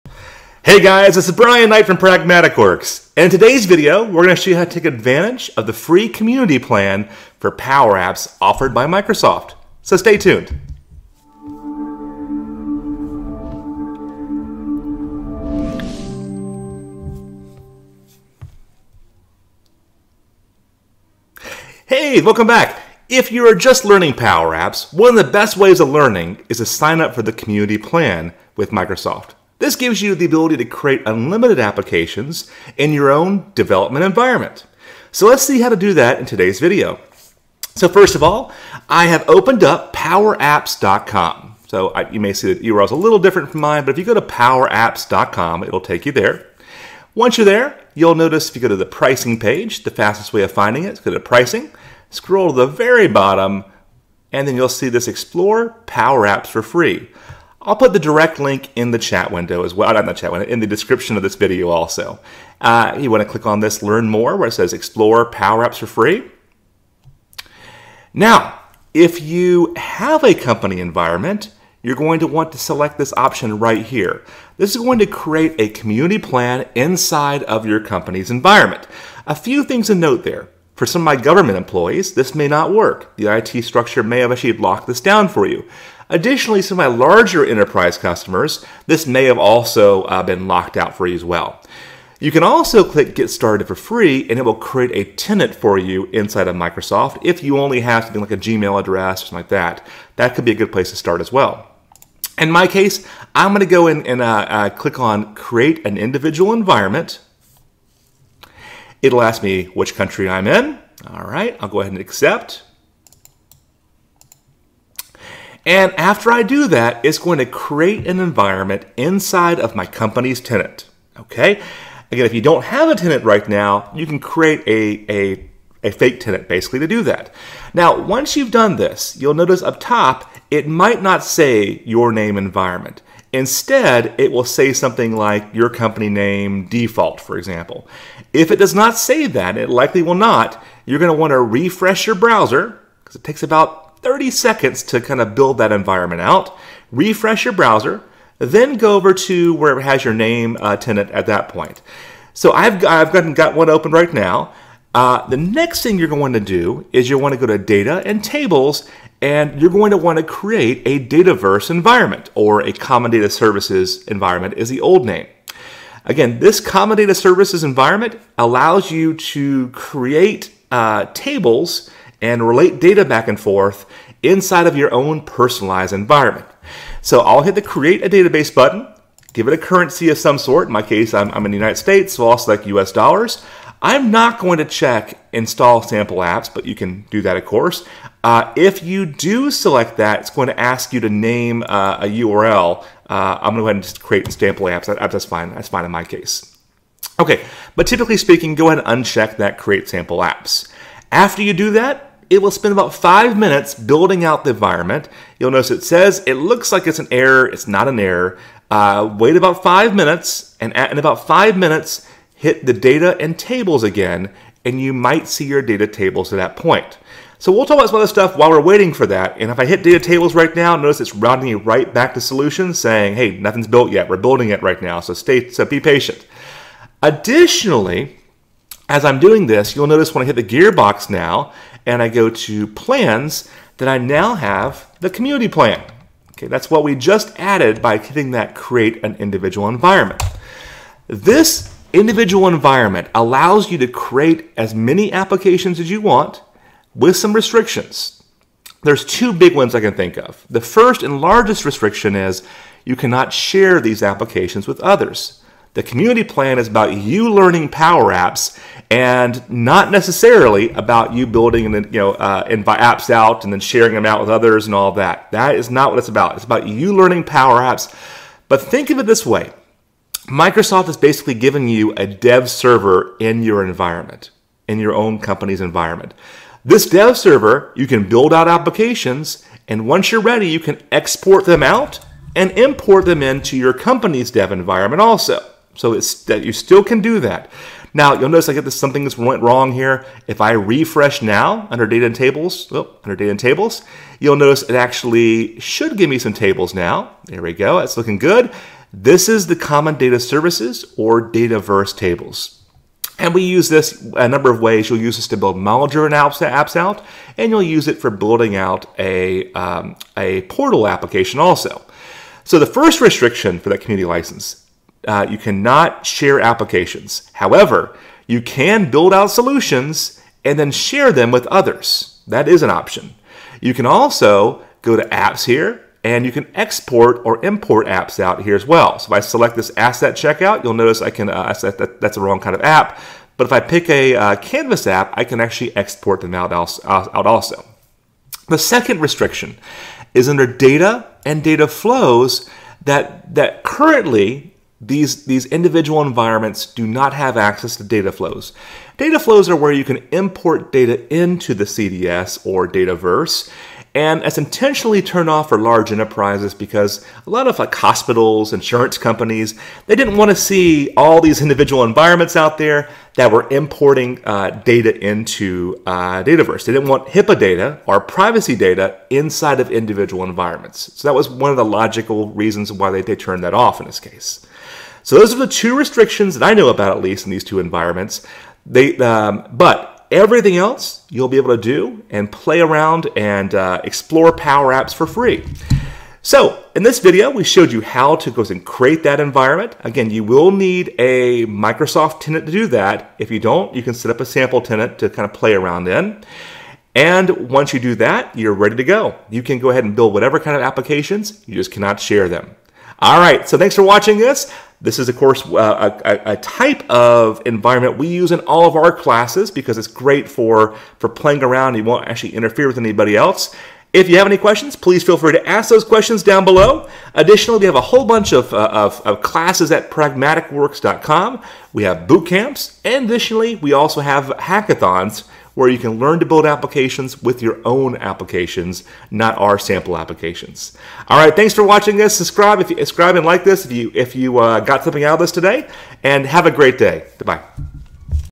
Getting Your Free Power Apps Developer Plan with Dataverse 💲
Learn how to get your forever free community plan of Power Apps and some of the restrictions behind that edition. You'll also see how to create a Dataverse (CDS / Common Data Services) database. Snag your free community license to learn Power Apps easier.